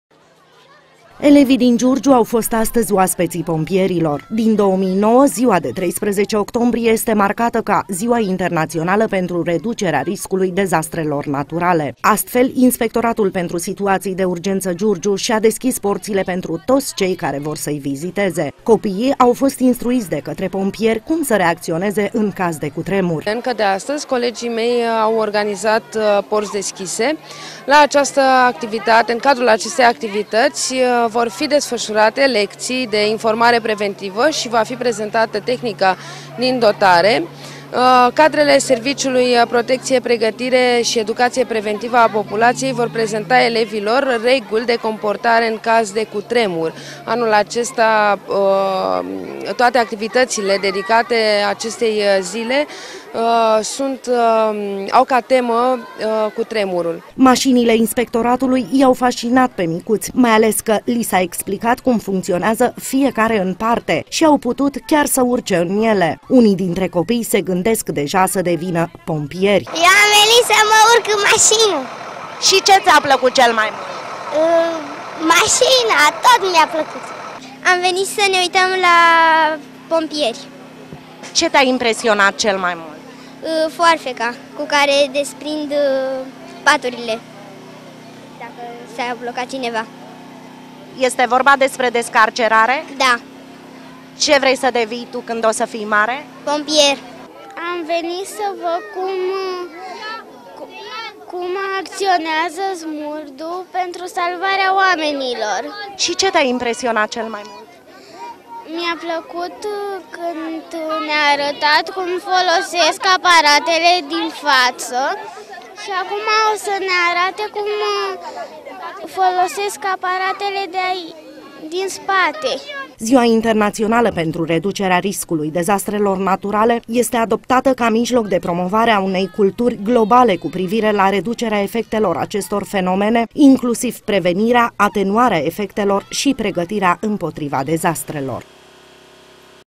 COPIII PREGATITI PENTRU CUTREMUR
Inspectoratul pentru Situații de Urgență și-a deschis porțile astăzi pentru 1.000 de copii de la școlile din Giurgiu. Așa a fost marcată Ziua Internațională pentru Reducerea Riscului Dezastrelor Naturale. Micuții au fost încântați de cele văzute, iar pe lângă asta au învățat cum să se comporte în caz de cutremur.